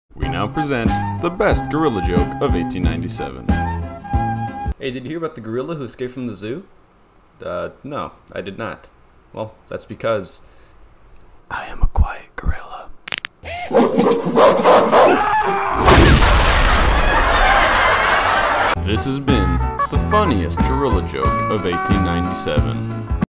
The Best Gorilla Joke of 1897
The Best Gorilla Joke of 1897 is now in animated form for easy digestion.